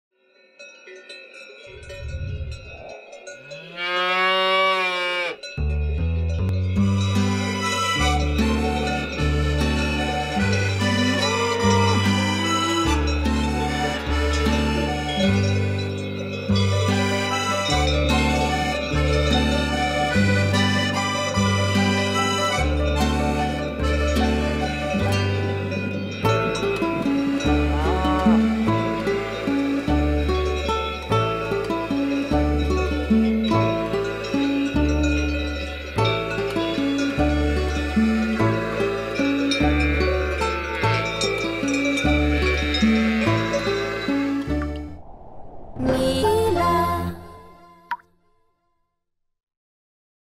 Zenzi's Muuhrakel: Russland - Saudi Arabien
Anlässlich der FIFA Fussball-Weltmeisterschaft in Russland sagt Milchkuh Zenzi, Südtirols WM-Orakel, den Ausgang der Spiele voraus.
In occasione della Coppa del Mondo FIFA in Russia, l'oracolo dell'Alto Adige Zenzi pronostica l'esito finale delle partite.